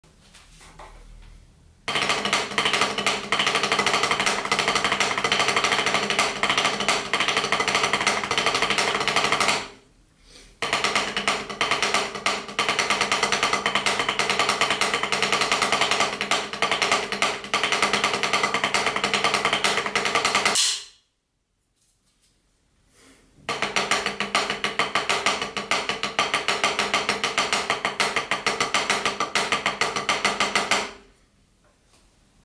paradiddle exercise
Useful paradiddle exercise: RRLRLLRLL RRLRLLRLL RRLRLR / LLRLRRLRR LLRLRRLRR LLRLRL